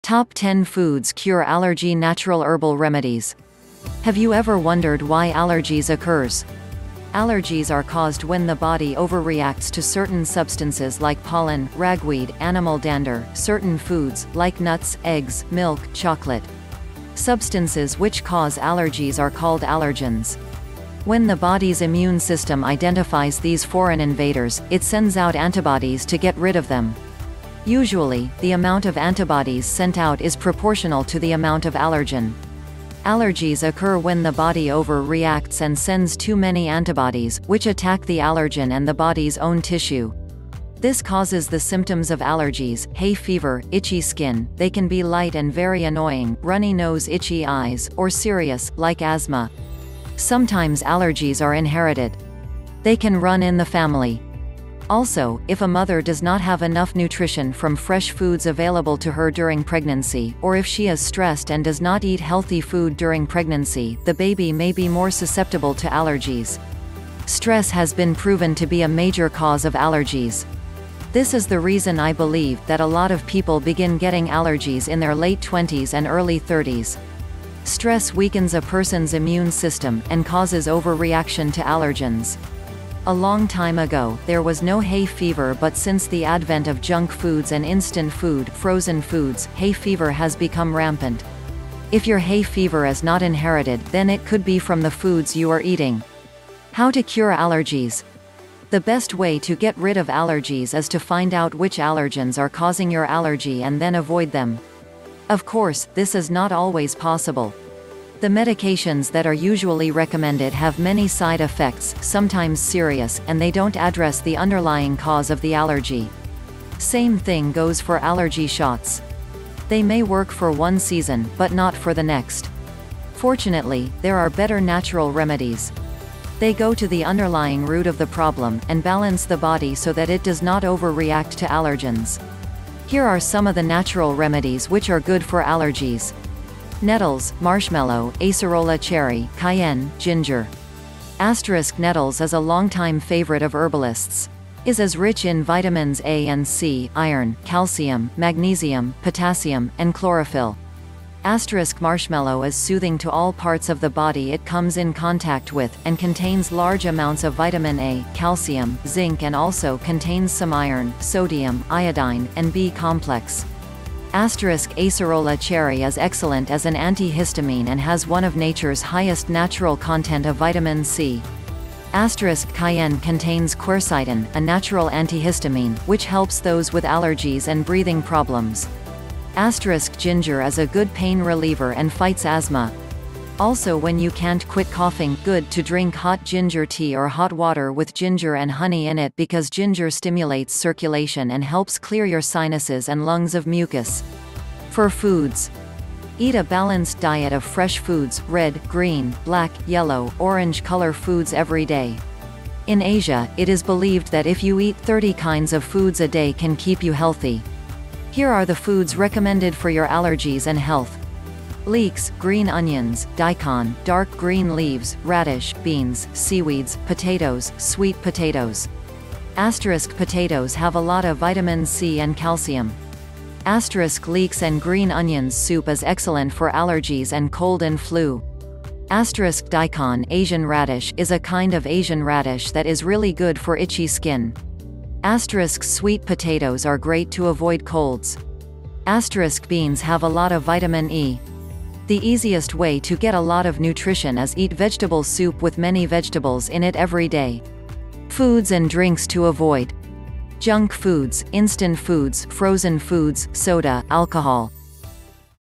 top 10 foods cure allergy natural herbal remedies 1
Top 10 foods cure allergy Natural Herbal  Remedies . Have you ever wondered why allergies occurs ?

Allergies are caused when the body over reacts to certain substances like pollen, ragweed, animal dander, certain foods, like nuts, eggs, milk, chocolate. Substances which cause allergies are called allergens.

When the body’s immune system identifies these foreign invaders, it sends out antibodies to get rid of them.

Usually, the amount of antibodies sent out is proportional to the amount of allergen. Allergies occur when the body over reacts and sends too many antibodies, which attack the allergen and the body’s own tissue.

This causes the symptoms of allergies, hay fever, itchy skin, they can be light and very annoying , runny nose itchy eyes, or serious, like asthma .

Sometimes allergies are inherited. They can run in the family. Also, if a mother does not have enough nutrition from fresh foods available to her during pregnancy, or if she is stressed and does not eat healthy food during pregnancy, the baby may be more susceptible to allergies.

Stress has been proven to be a major cause of allergies. This is the reason I believe, that alot of people begin getting allergies in their late 20’s and early 30’s. Stress weakens a person’s immune system, and causes over reaction to allergens.

A long time ago, there was no hay fever but since the advent of junk foods and instant food ( frozen foods), hay fever has become rampant. If your hay fever is not inherited , then it could be from the foods you are eating.

How to cure allergies ?

The best way to get rid of allergies is to find out which allergens are causing your allergy and then avoid them.

Of course, this is not always possible.

The medications that are usually recommended have many side effects , sometimes serious, and they don’t address the underlying cause of the allergy.

Same thing goes for allergy shots. They may work for one season, but not for the next.

Fortunately, there are better natural remedies. They go to the underlying root of the problem, and balance the body so that it does not over react to allergens.

Here are some of the natural remedies which are good for allergies.

Nettles, Marshmallow, Acerola Cherry, Cayenne, Ginger.

* Nettles is a longtime favorite of herbalists. is is rich in vitamins A and C, iron, calcium, magnesium, potassium, and chlorophyll.

* Marshmallow is soothing to all parts of the body it comes in contact with, and contains large amounts of vitamin A, calcium, zinc and also contains some iron, sodium, iodine, and B-complex.

* Acerola Cherry is excellent as an anti-histamine and has one of nature’s highest natural content of Vitamin C.

* Cayenne contains quercitin, a natural antihistamine, which helps those with allergies and breathing problems.

* Ginger is a good pain reliever and fights asthma . Also when you can’t quit coughing, good to drink hot ginger tea or hot water with ginger and honey in it because ginger stimulates circulation and helps clear your sinuses and lungs of mucus.

For foods,

Eat a balanced diet of fresh foods, red, green, black, yellow, orange color foods every day. In asia, it is believed that if you eat 30 kinds of foods a day can keep you healthy.

Here are the foods recommended for your allergies and health,

Leeks, green onions, Daikon, dark green leaves, Raddish, beans, seaweeds, potatoes, sweet potatos.

* Potatos have alot of Vitamin C and calcium .

* Leeks and green onions soup is excellent for allergies and cold and flu.

* Daikon (asian raddish) is a kind of Asian raddish that is really good for itchy skin

* Sweet potatoes are great to avoid colds.

* Beans have alot of Vitamin E .

The easiest way to get a lot of nutrition is eat vegetable soup with many vegetables in it every day.

Foods and drinks to avoid

Junk foods , instant foods ( frozen foods ) , soda, alcohol.